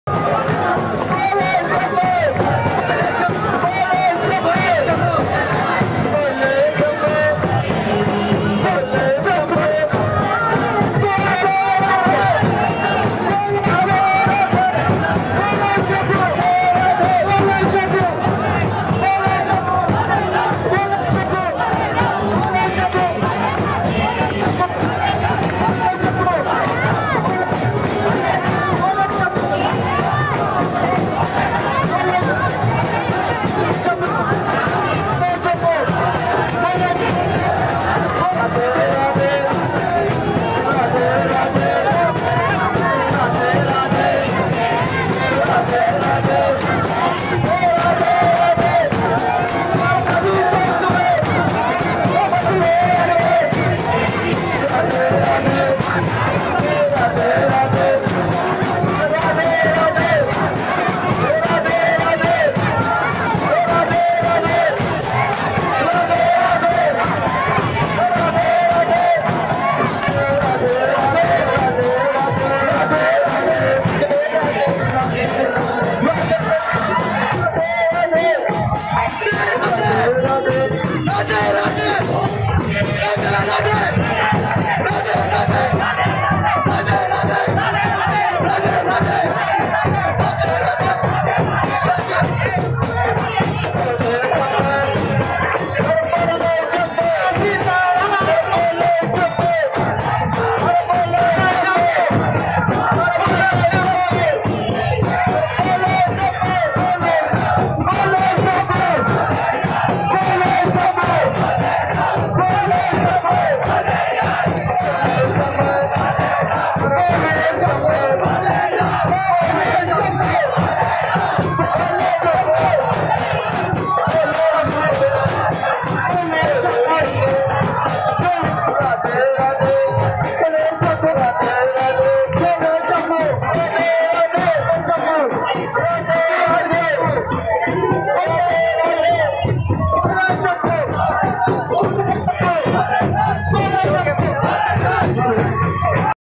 Jal jhulni ekadashi, Madhav Chauk, Soyat Kalan
Maha Aarti of "BEWAN" at Madhav Chauk, Soyat Kalan